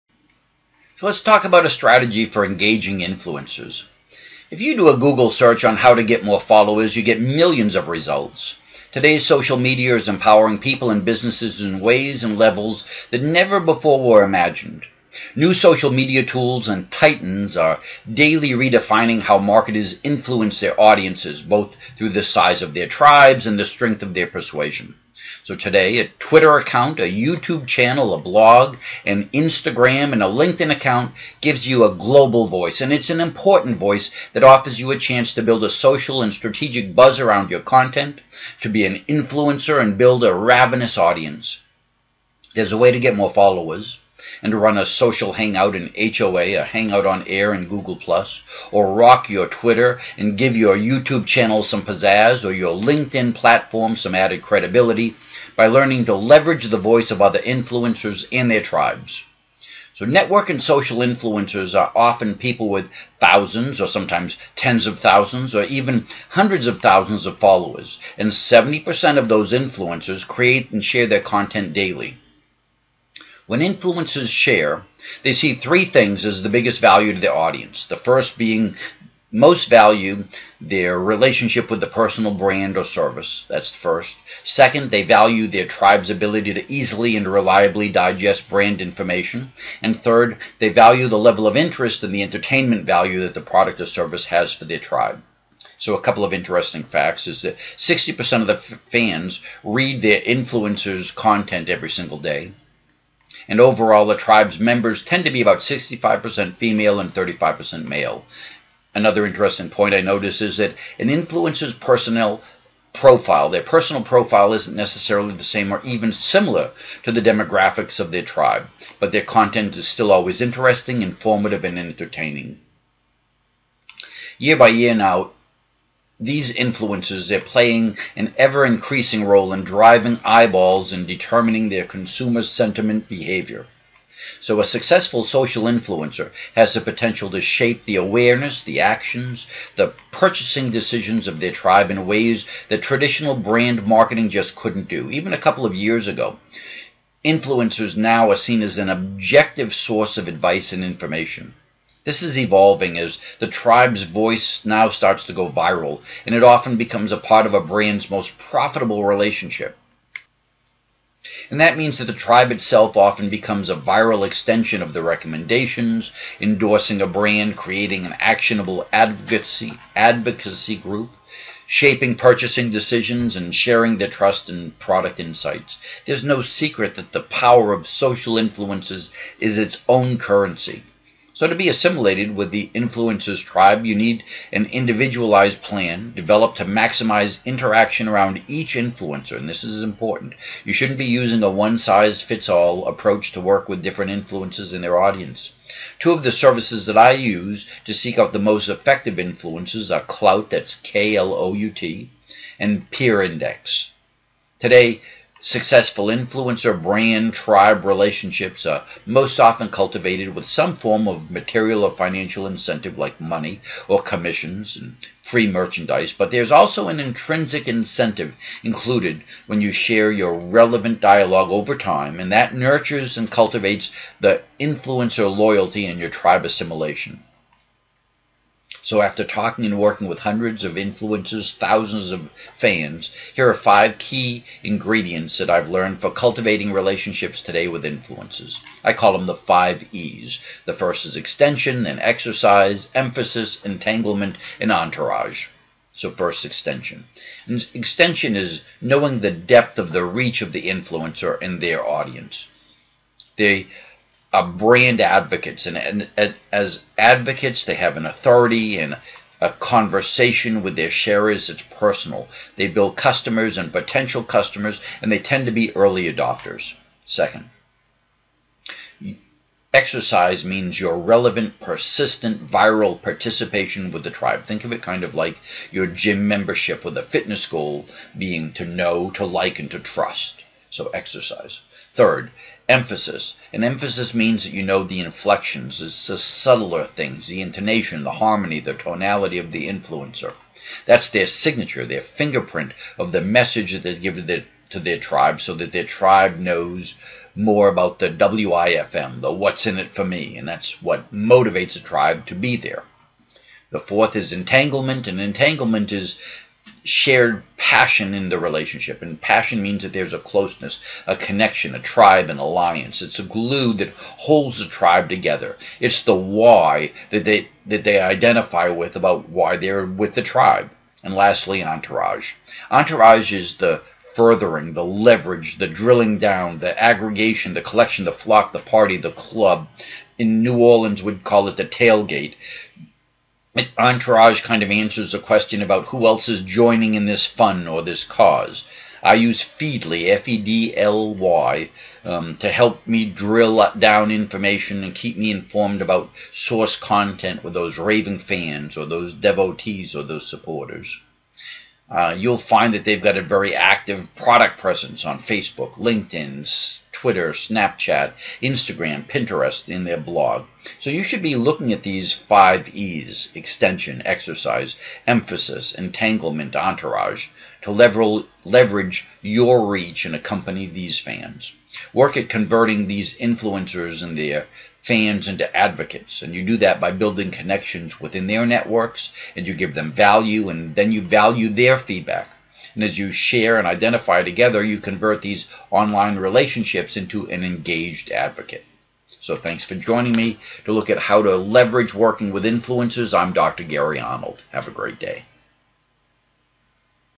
A Strategy For Leveraging Influencers by Dr Gary Arnold
Be an Influencer and build a ravenous audience.   Here’s a way to get more followers, run a social hangout, rock your Twitter feed, give your YouTube channel some pizzazz or your LinkedIn platform added credibility by learning to leverage the voice of other Influencers and their tribes.